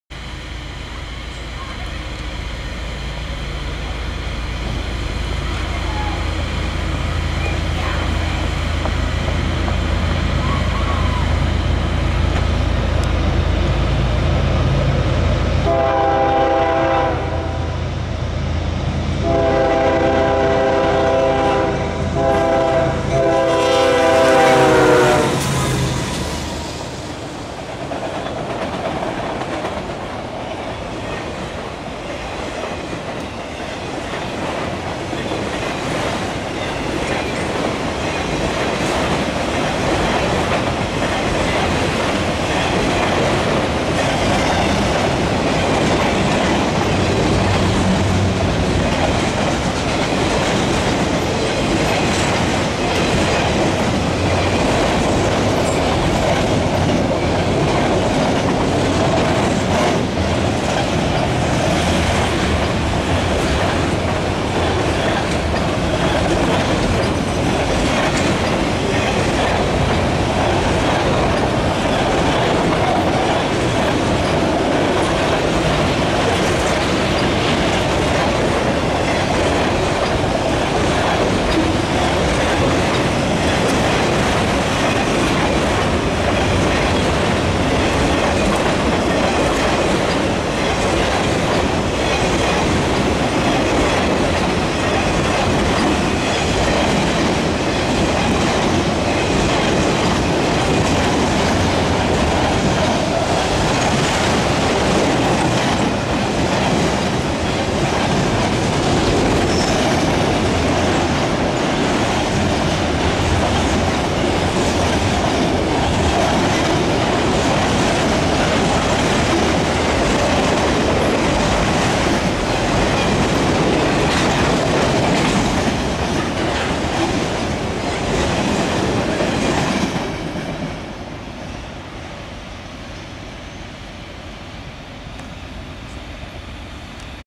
CSX Q217-02 AGAIN!!
The surprise at Gaithersburg is Q217-02, the same one!! We beat him to Gaithersburg, MD and caught him again! Isn't a rare!? This is awesome, same engines, same cars.